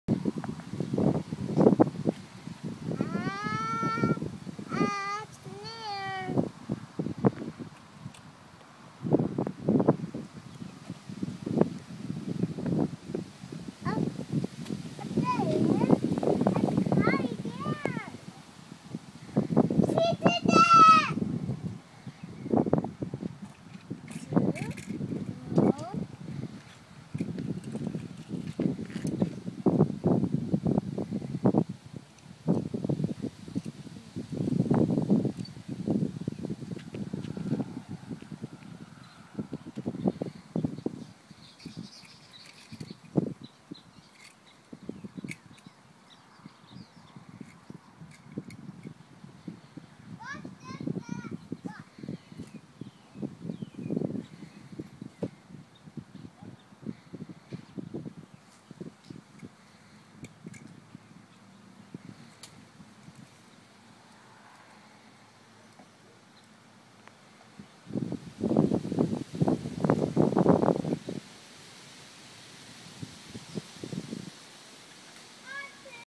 Playing at the park
Carter playing at the park, chooses to play cars on a picnic table instead of playing on the swings and slides.